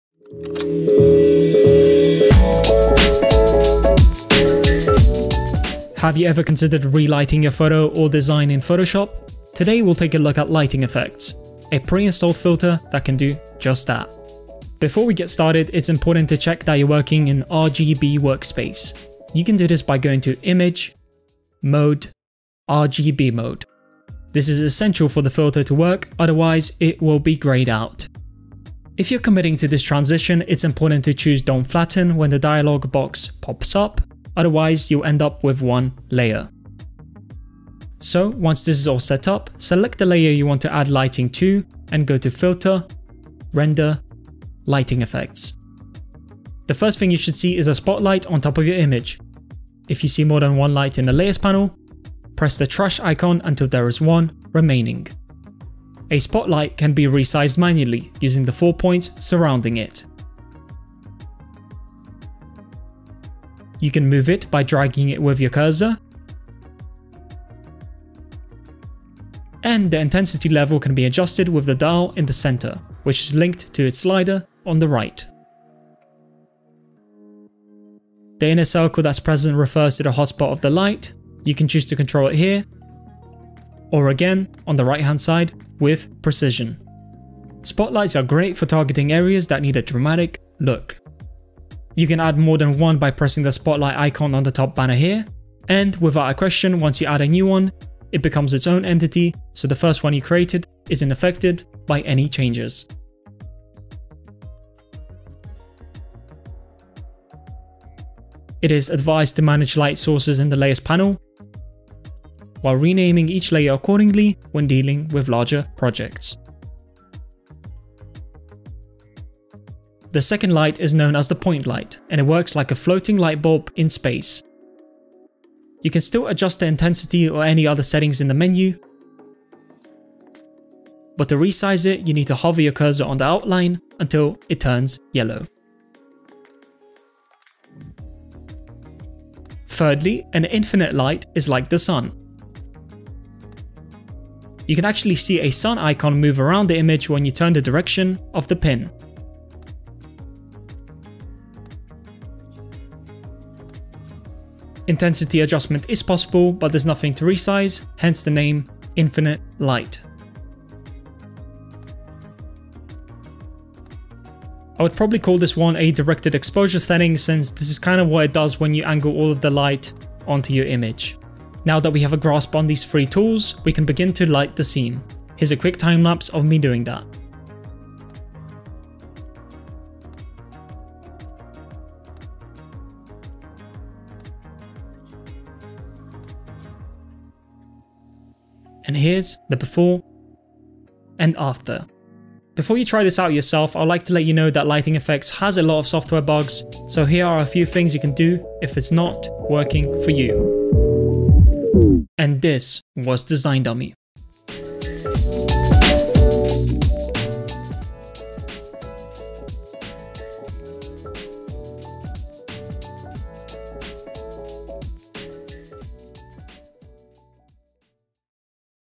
Relight Anything using Lighting Effects in Photoshop
You will learn how to re-light photos in Photoshop using the in-built feature called 'Lighting Effects. This effect can be used to drastically change the mood and atmosphere of your images.